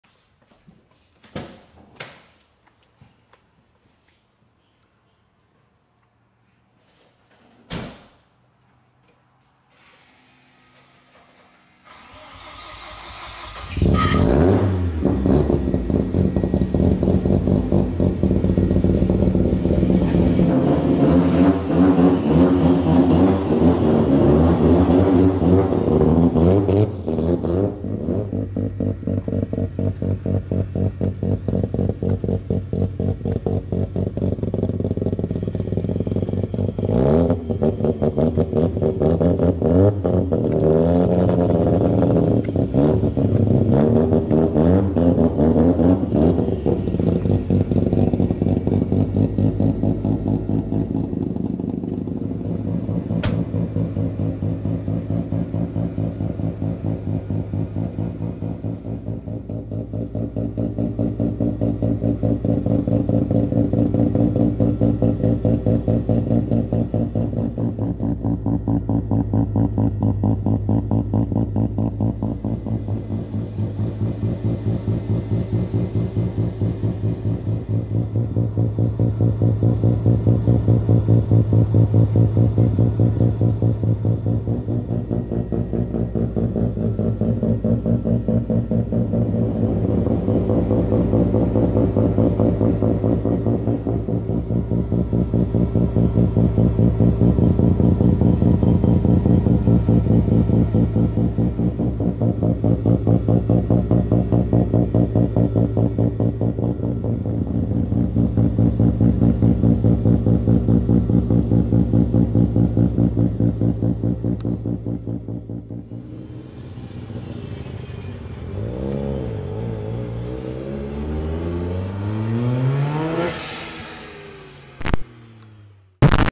Rotary Idle ASMR | TJ Hunt's 2-Rotor RX7 [4K]
Just sit and listen..

There would be driving audio in this too but my new fancy audio recorder that I've been saving for years for wasn't working :/ Gonna finally listen to the 1000 comments per video about wanting more car audio hahaha.. but I will say its a lot more work than you might think to make a car video with only raw audio.. at least a good one that doesn't break the 4th wall..

Download all my footage and watch exclusive behind the scenes content

Equipment and Tools I use.